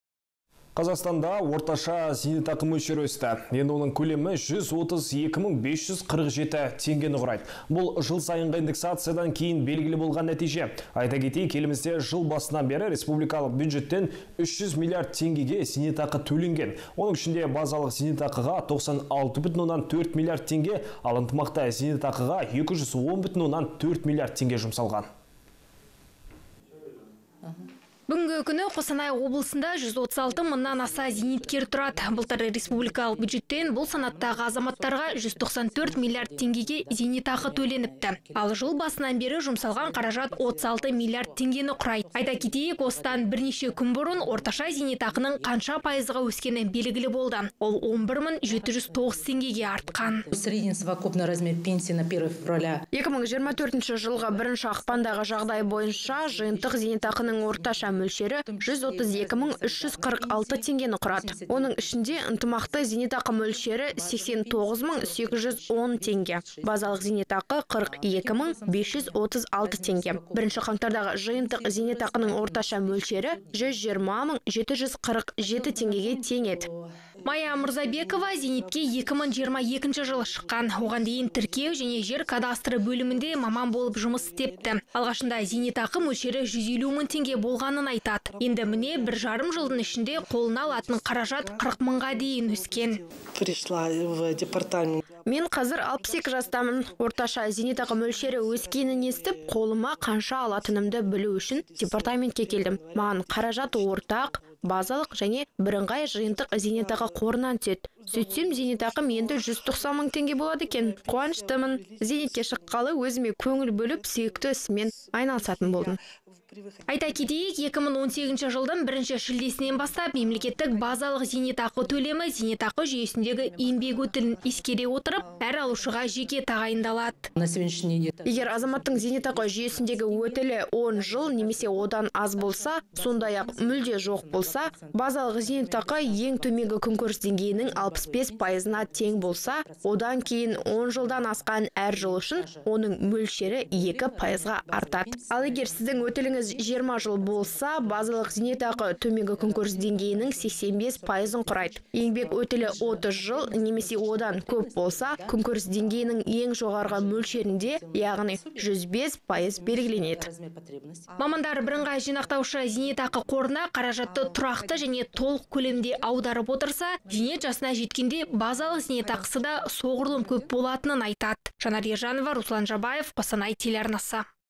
Қазақстанда орташа зейнетақы өсті
Қазақстанда орташа зейнетақы мөлшері өсті. Енді оның көлемі 132 547 теңгені құрайды. Бұл жыл сайынғы индексациядан кейін белгілі болған нәтиже. Айта кетейік, елімізде жыл басынан бері республикалық бюджеттен 300 млрд теңгеге зейнетақы төленген. Оның ішінде базалық зейнетақыға 96,4 млрд теңге, ынтымақты зейнетақыға 210,4 млрд теңге жұмсалған.